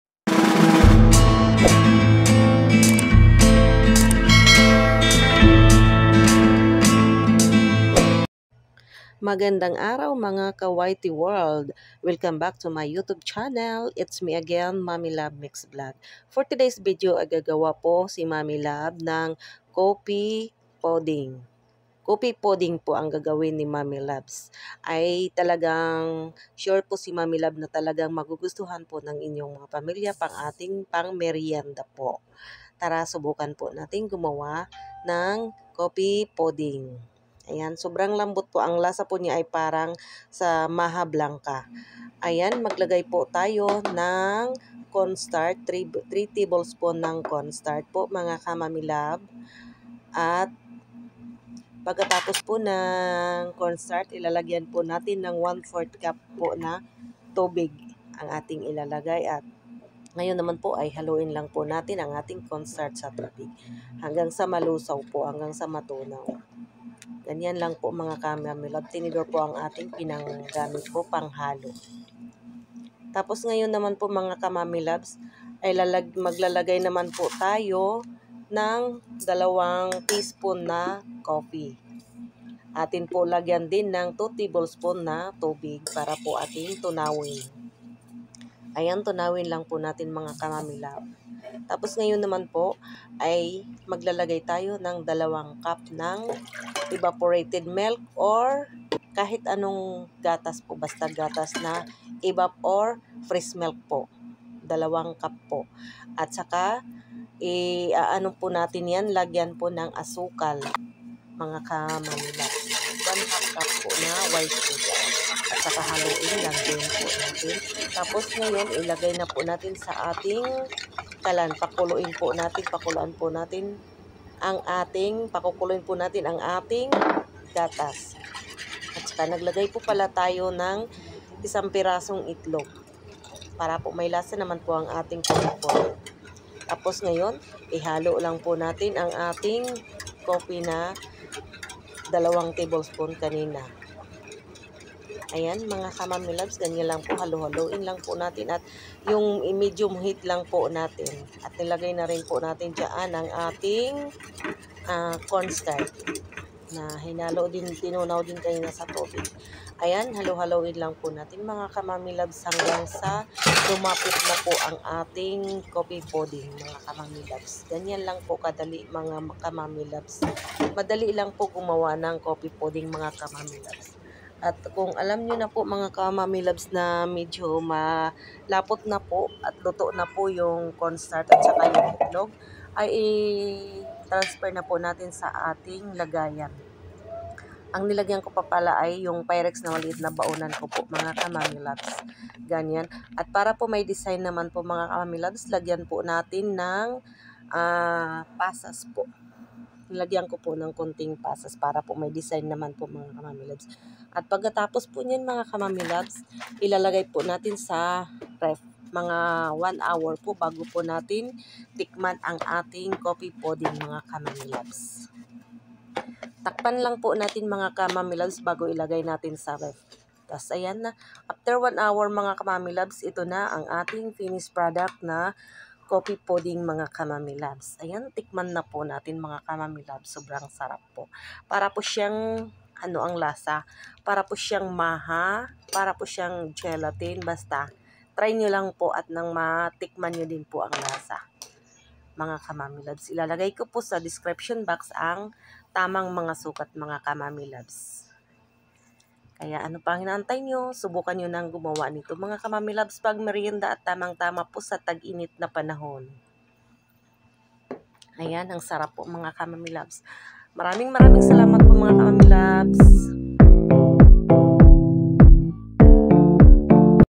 COFFEE PUDDING NO NEED OVEN
COFFEE PUDDING DESSERT NO OVEN

3      TBSP Corn flour
1/4  cup water
2      TSP instant coffee
2      TBSP water
2      cup milk
1/2  cup sugar
1      egg
boil milk at ilagay ang cornstart sunod ang egg sunod ang coffee at ilagay sa ref 40 mnit to 1hour